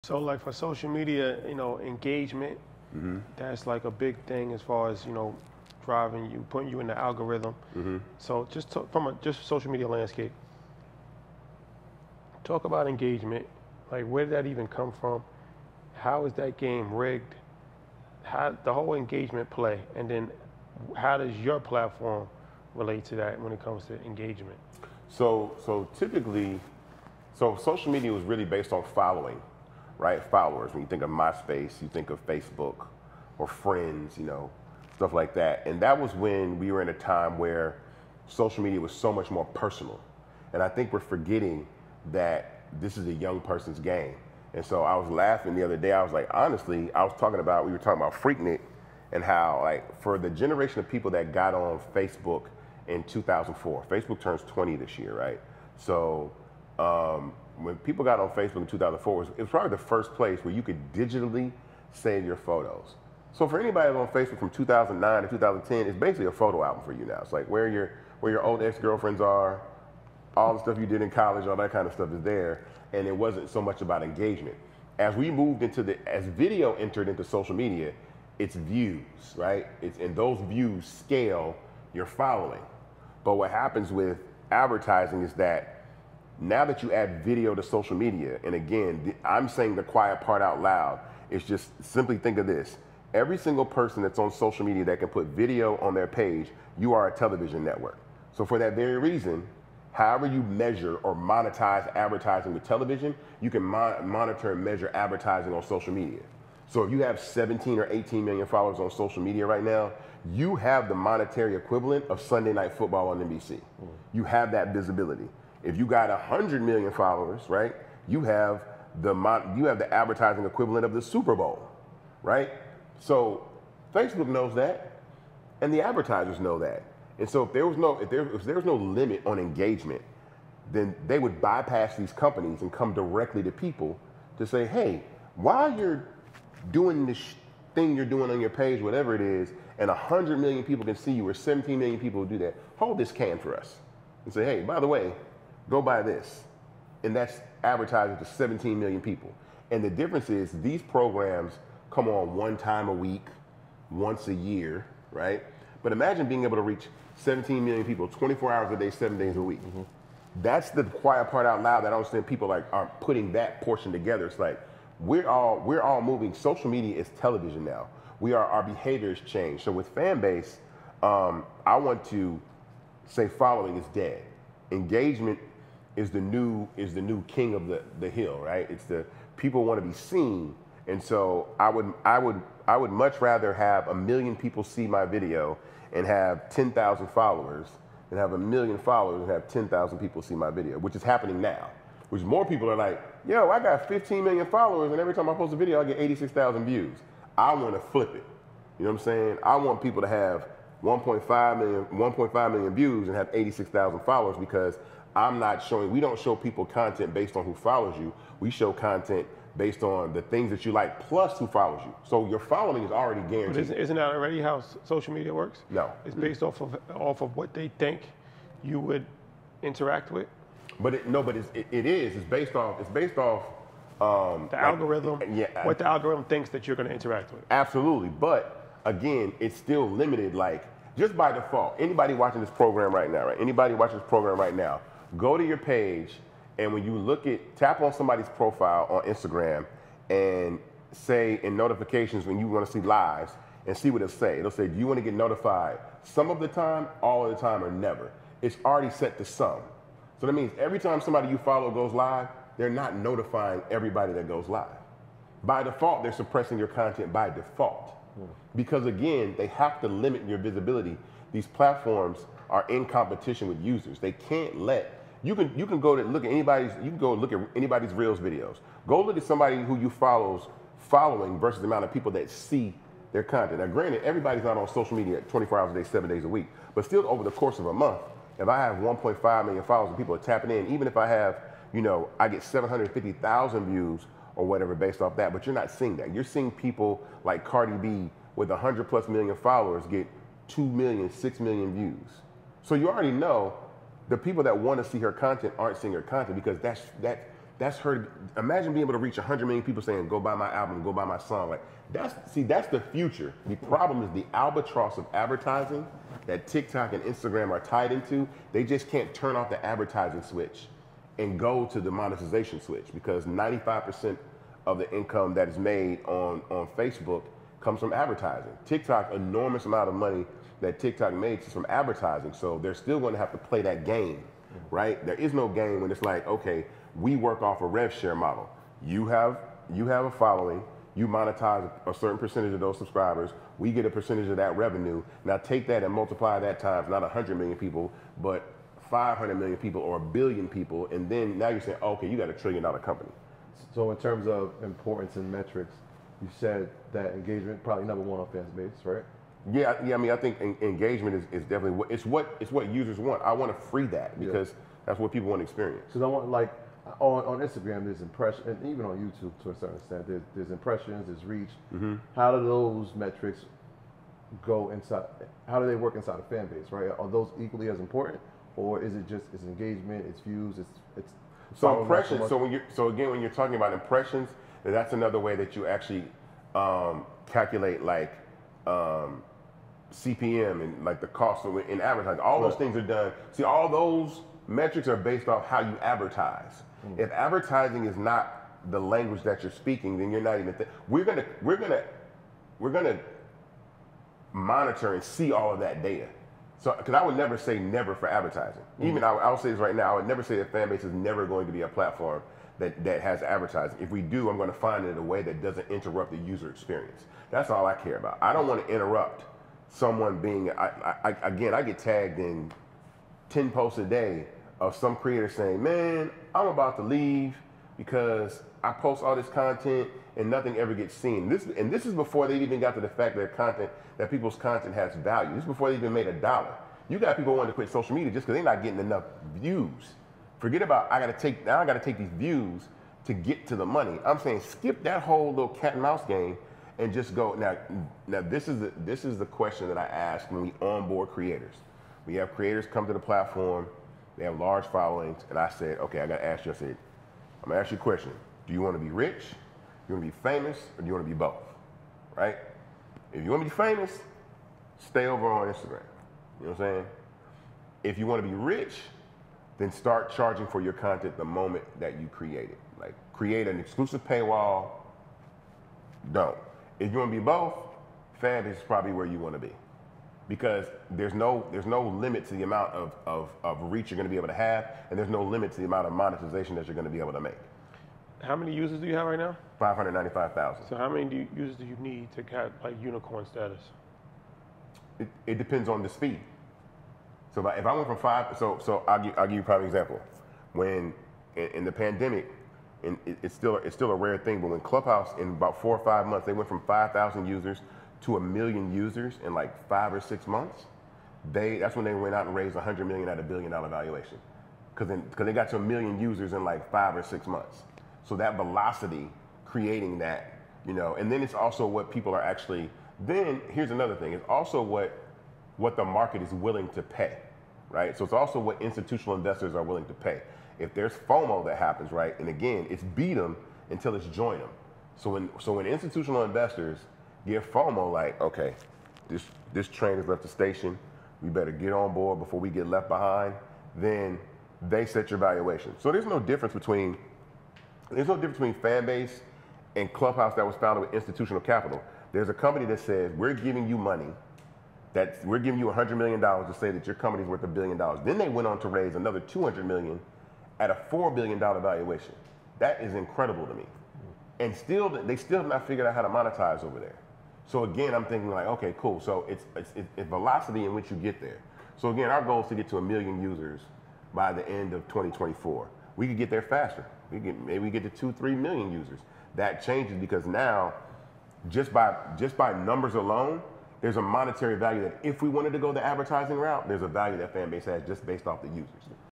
Social Media is Becoming the New TV
Explore the pivotal insights on social media's evolution from its personal origins to a powerful advertising medium, mirroring television's reach, as discussed with Isaac Hayes III. Dive deep into the intricacies of engagement, followership, and the impact of algorithms on content visibility. Learn how platforms like Fanbase redefine engagement, prioritizing authentic reach over sheer follower count and presenting creators with the choice between fame, fortune, or both. With 595,000 users on Fanbase, Isaac Hayes III is navigating towards a new horizon of user interaction and monetization, shaping a future where content creators have unlimited potential for reach and earnings.